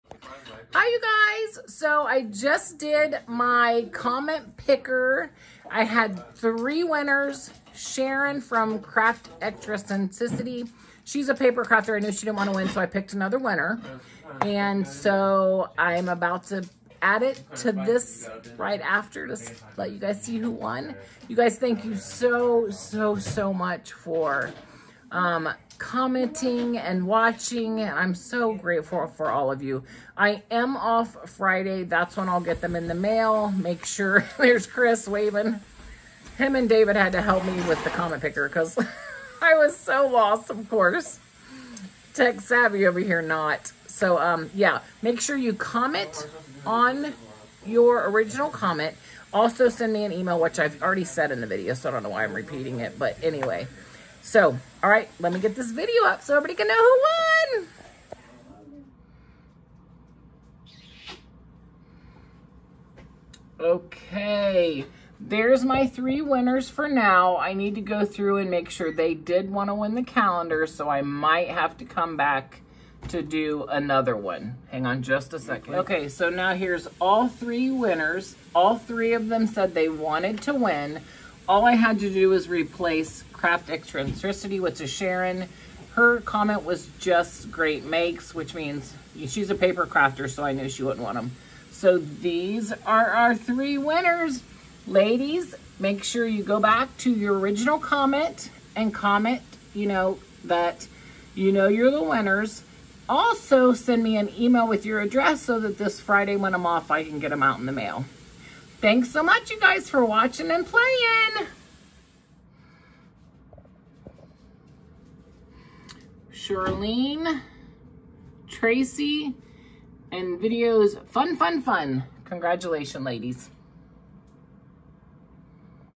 Dollar Tree Calendars Giveaway Announce
Hey to all my crafty peeps here are my three winners.

Ladies you have 72 hours to get a hold of me for your calendars or I will draw other winners. Friday by 6pm EST

Tracey has gotten a hold of me. Waiting on two more.